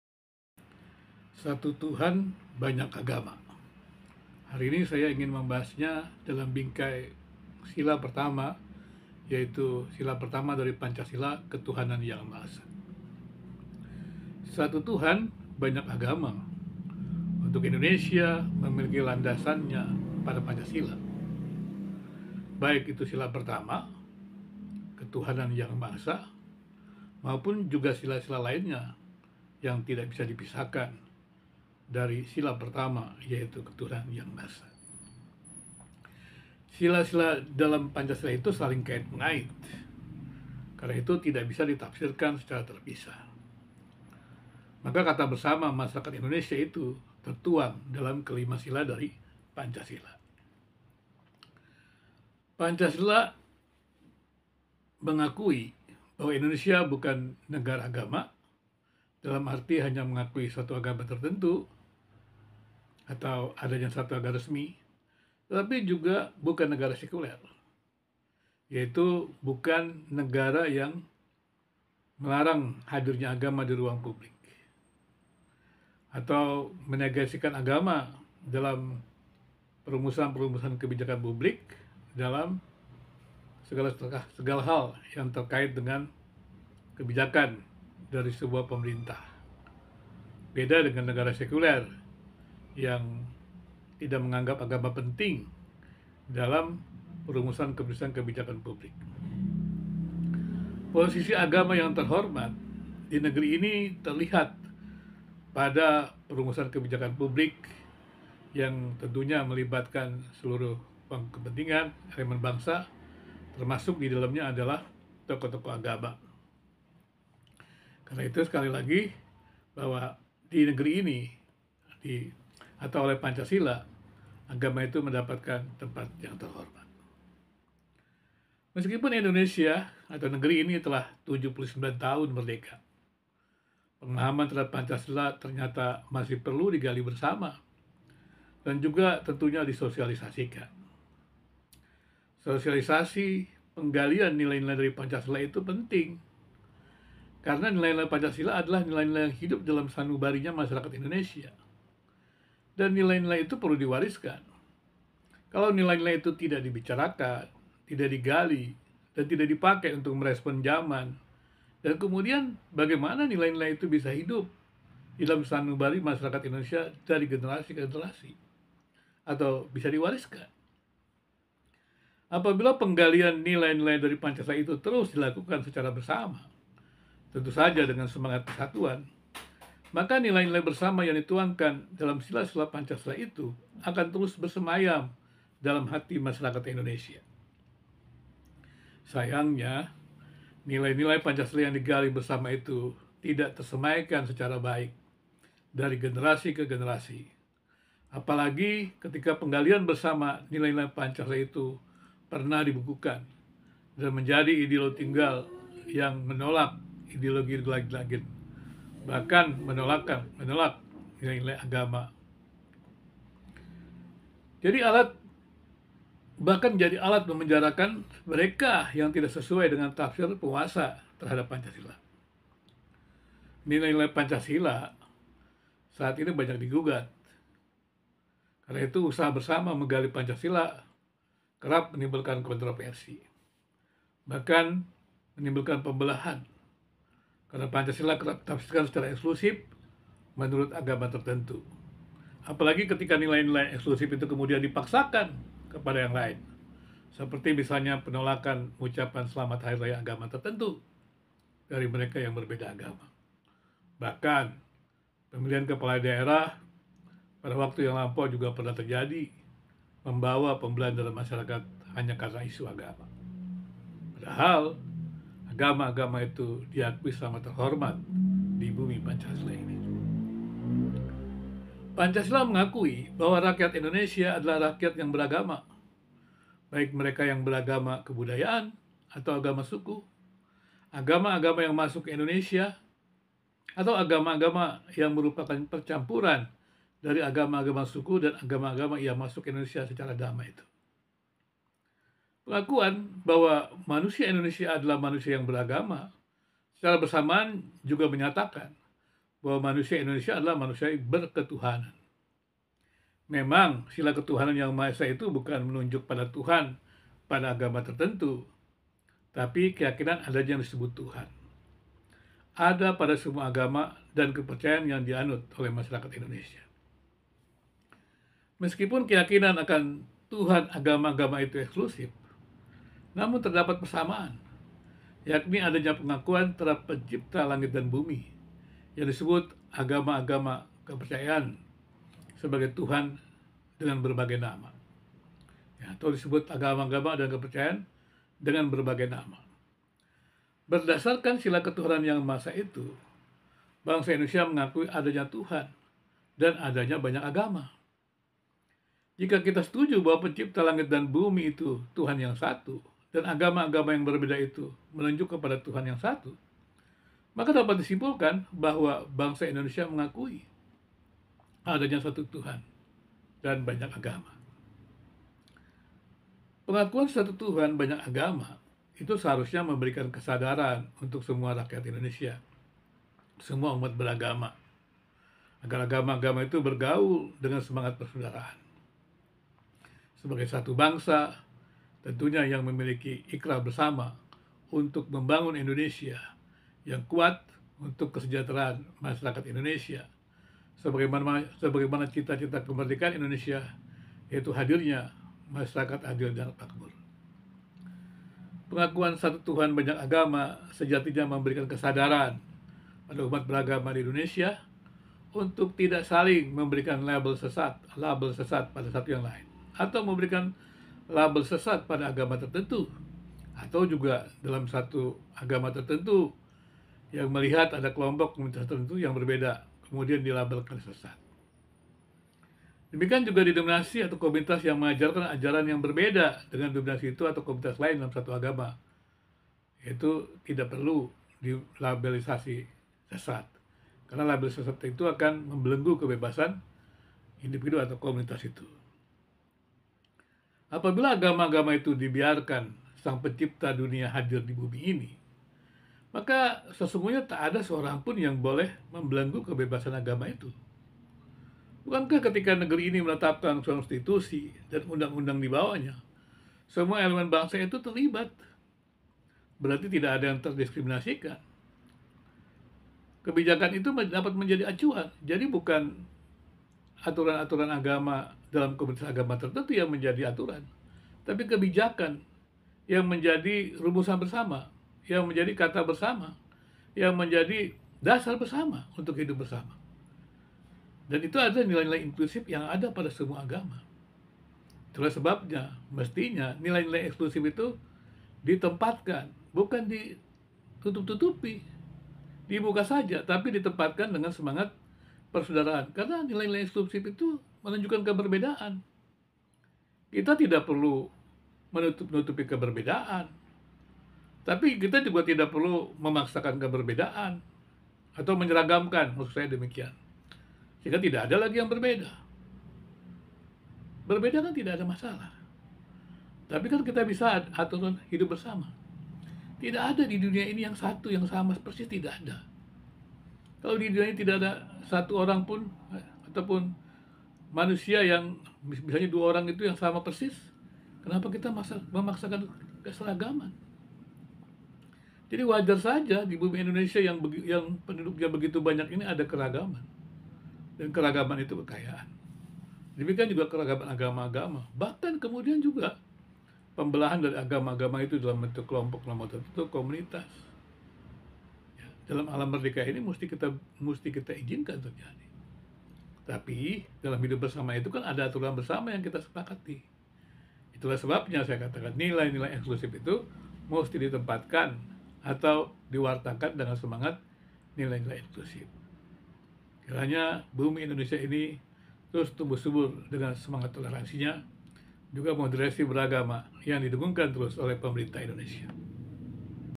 Controversial Topic: The One God, Many Religions
Explore the controversial topic of the one God and many religions in this thought-provoking video. Learn about religious tolerance and human rights.
Pluralisme, Satu Tuhan banyak agama menurut Pancasila, sila ketuhanan yang maha esa perlu dipahami dengan baik untuk menguatkan toleransi beragama di Indonesia.